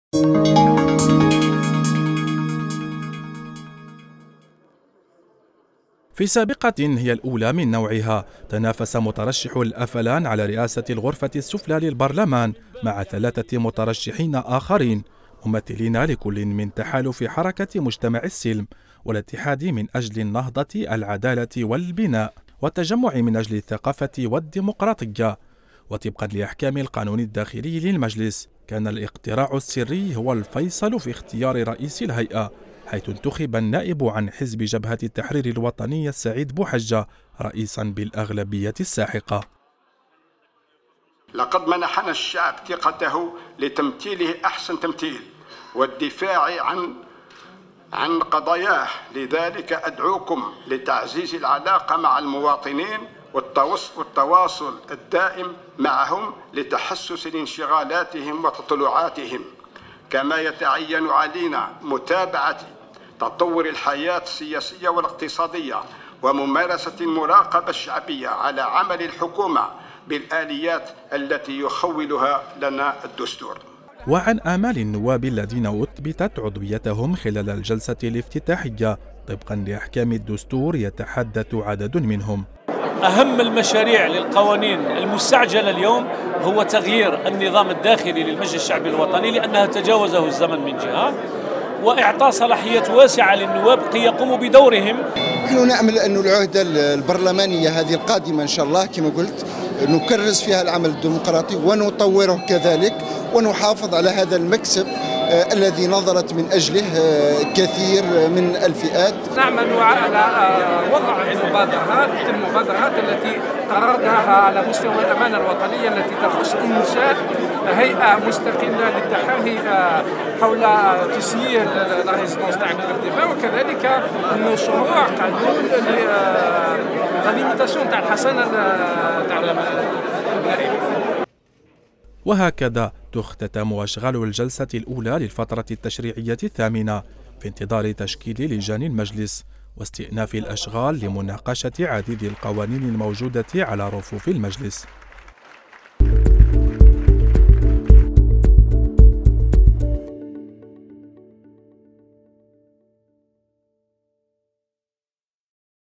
السعيد بوحجة يفوز برئاسة المجلس الشعبي الوطني أمام ثلاثة منافسين
الجزائر (وأج) – في سابقة هي الأولى من نوعها، تنافس أربعة مترشحين على رئاسة المجلس الشعبي الوطني، في الفترة التشريعية الثامنة ، وفاز مترشح حزب جبهة التحرير الوطني السعيد بوحجة، على منافسيه من تحالف حركة مجتمع السلم، والاتحاد من أجل النهضة والعدالة والبناء، والتجمع من أجل الثقافة والديمقراطية بالأغلبية الساحقة.